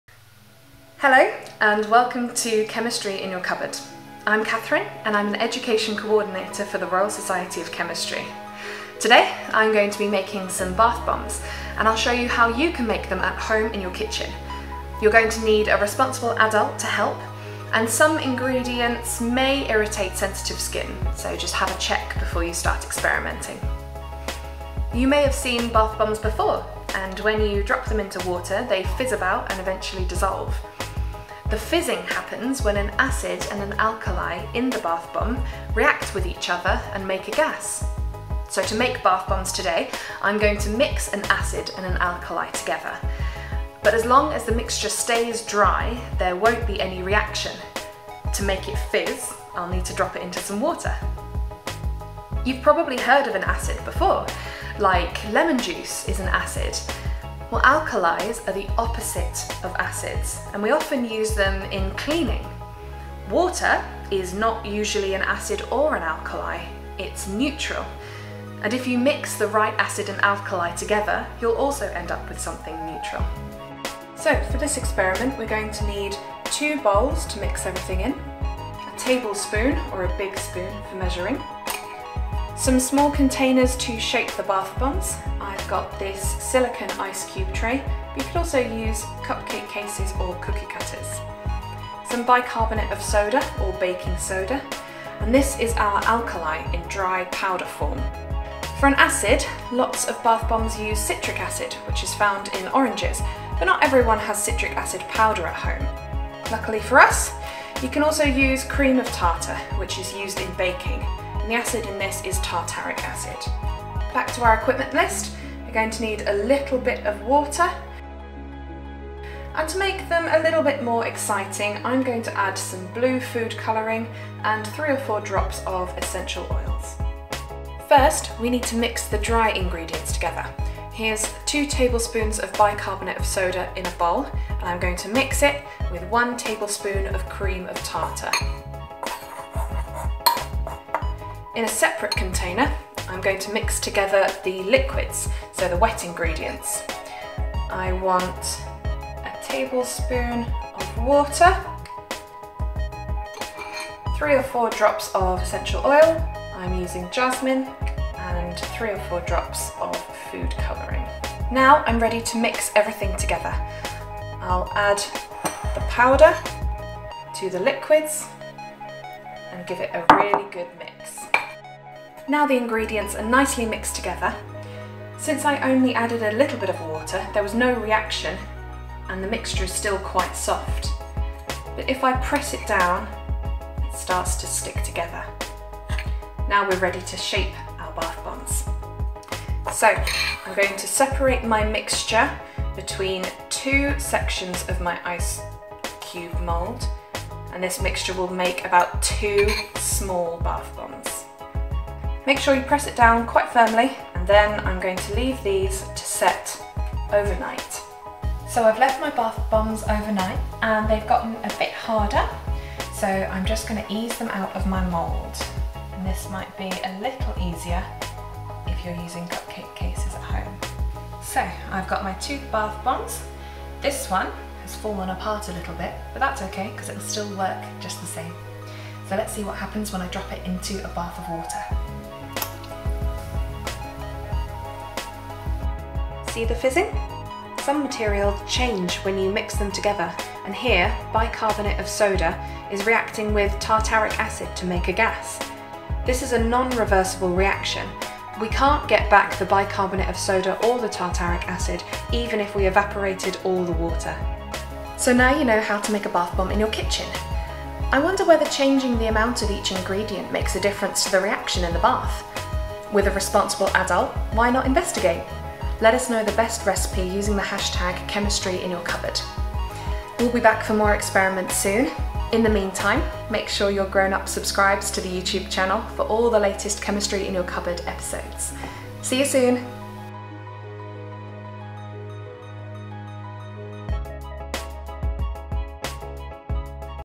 Making Bath bombs
Did you know you can make bath bombs using ingredients you can find in your kitchen?

RSC Education Coordinator Catherine shows us how to make fun fizzing bath bombs by making a mixture of an acid and an alkali and explaining the reaction when they are dropped into a bath

What you need:

- Two bowls
- A tablespoon
- Ice cube box, or something to shape the bath bombs
- Bicarbonate of soda
- Citric acid powder or cream of tartar
- A small amount of water
- Food colouring
- Essential oils

Have a go yourself and don’t forget to share your results with us ChemistryInYourCupboard​